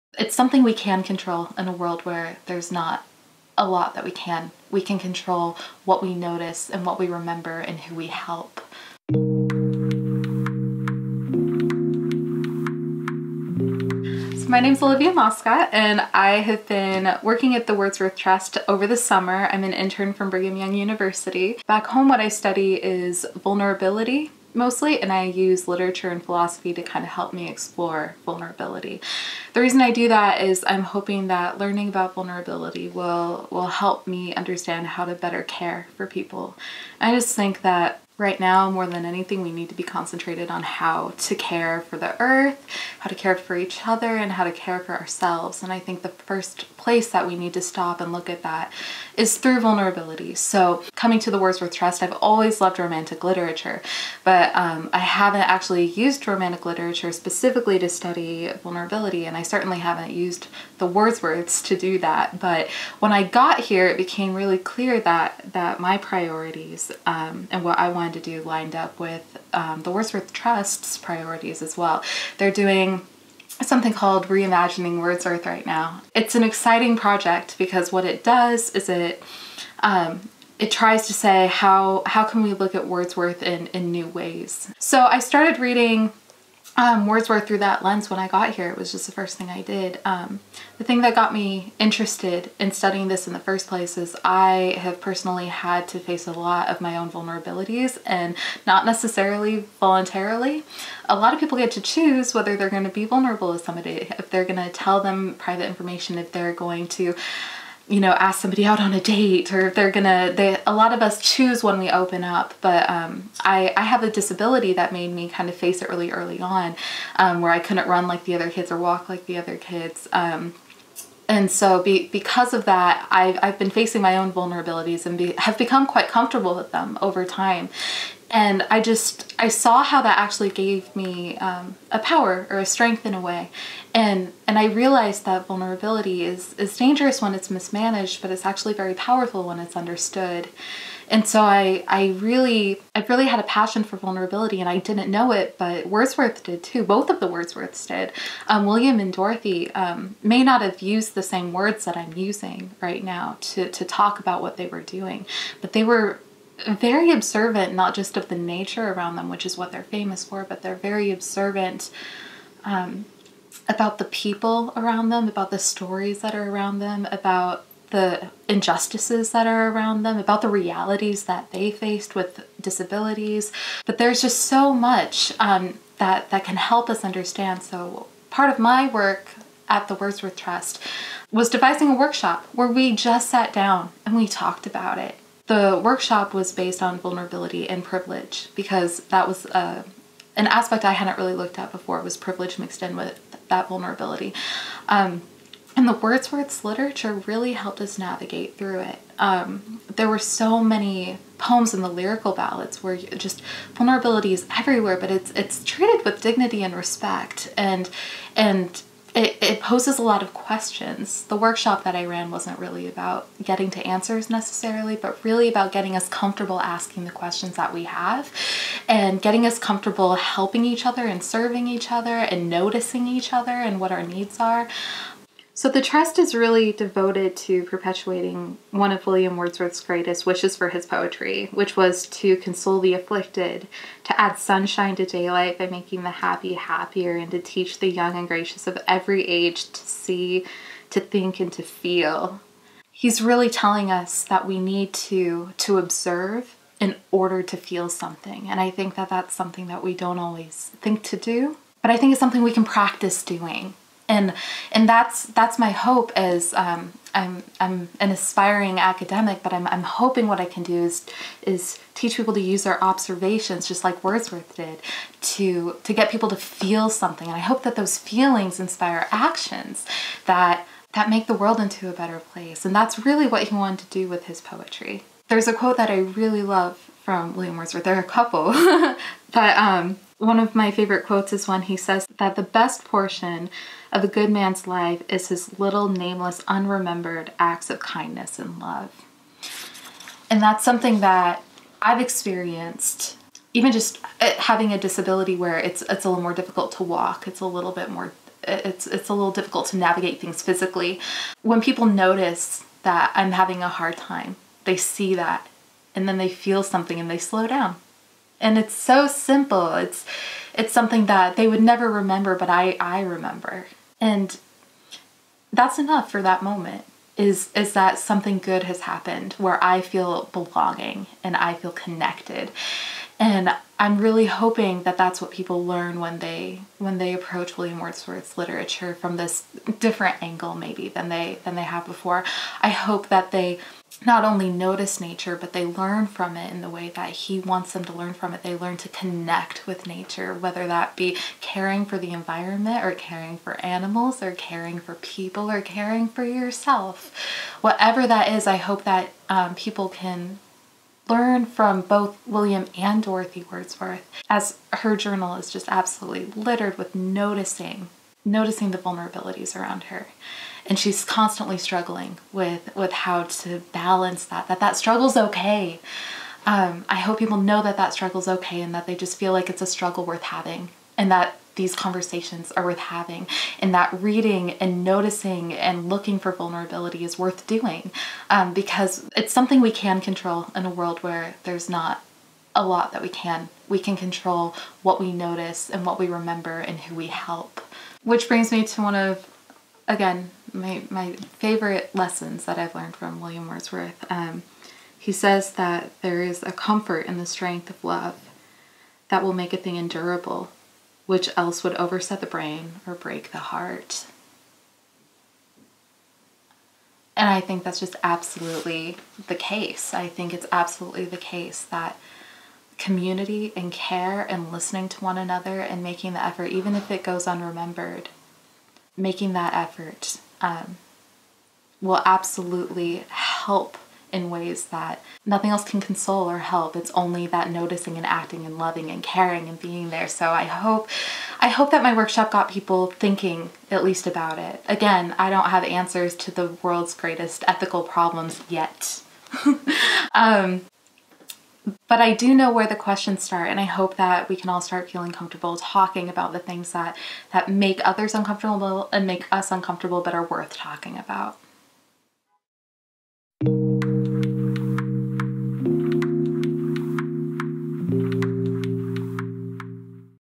Wordsworth & Vulnerability // New Perspectives
Over the summer, Olivia (intern from Brigham Young University) has been exploring how the writings of William and Dorothy Wordsworth can help us talk about vulnerability.